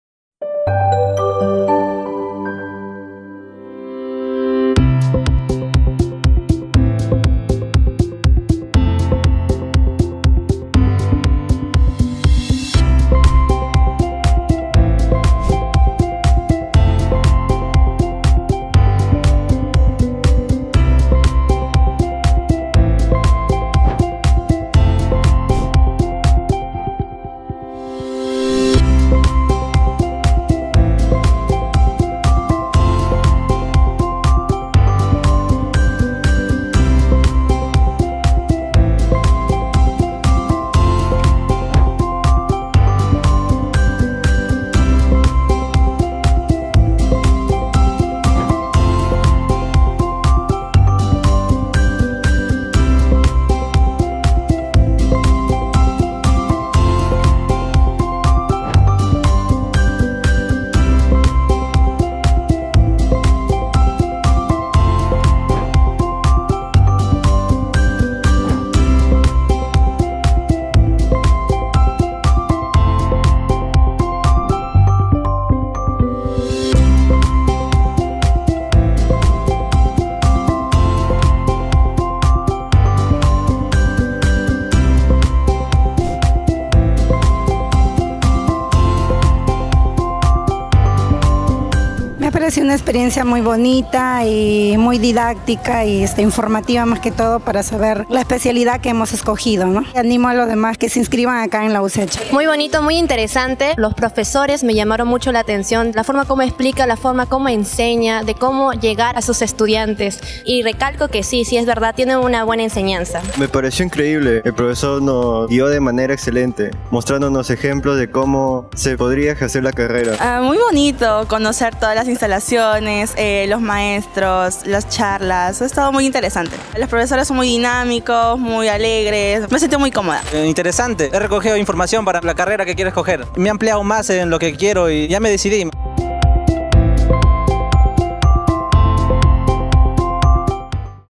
Un Día en la UCH: Primera Edición 2023-I
¡Un éxito rotundo! Centenares de futuros profesionales visitaron nuestro campus y vivieron la experiencia de estar UnDíaEnLaUCH. Si te lo perdiste, tendremos una nueva edición este sábado 18 de marzo en donde conocerás nuestras 15 carreras profesionales y serás parte de talleres vivenciales, activaciones y mucho más.
¡Te esperamos!
📍 Campus UCH : Av. Universitaria 5175, Los Olivos